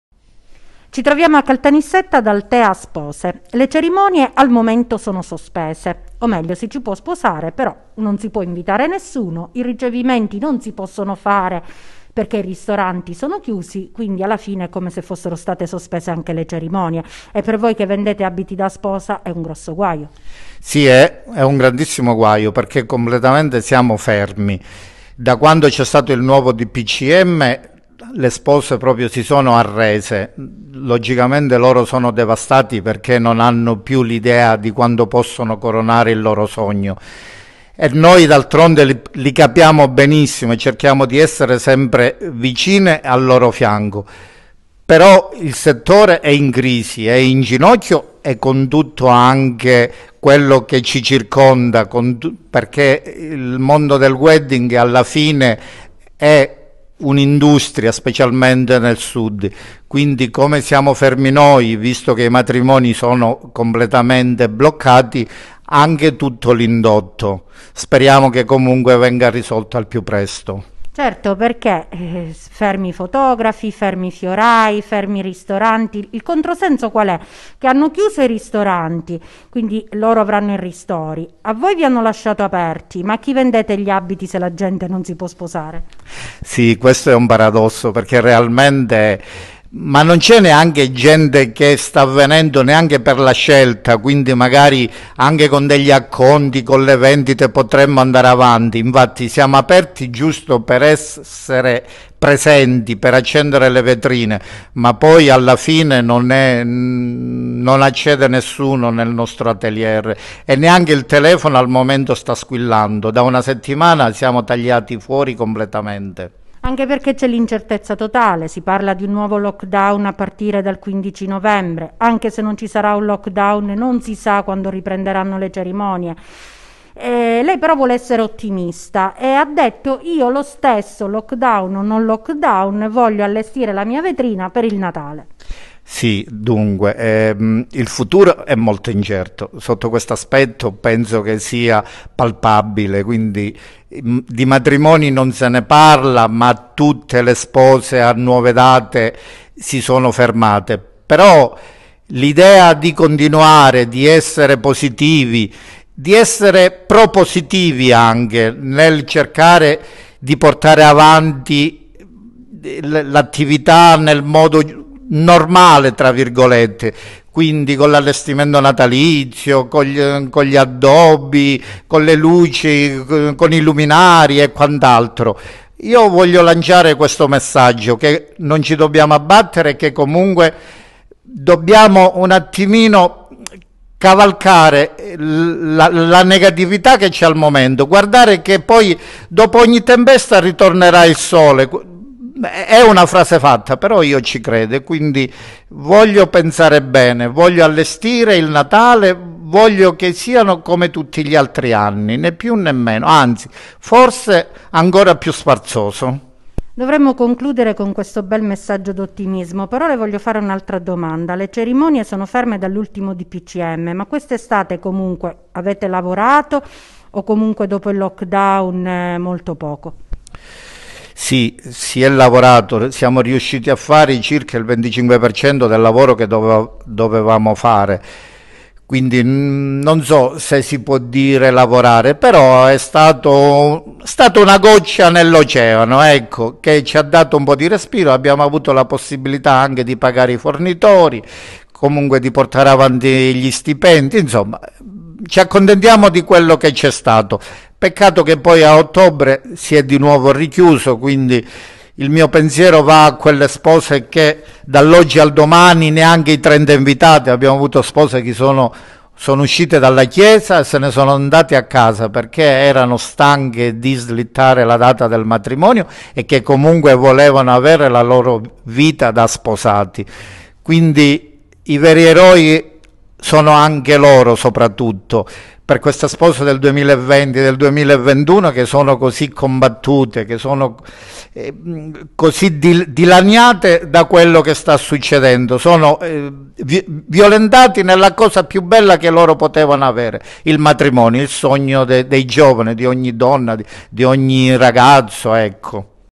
Cerimonie sospese, Althea: “Alle spose viene negato il sogno più bello, il matrimonio”
Cerimonie sospese e gli atelier sono in ginocchio. Già questa estate hanno avuto il 25% del fatturato base, da ottobre con i matrimoni annullati e gli abiti della nuova collezione acquistati è un disastro.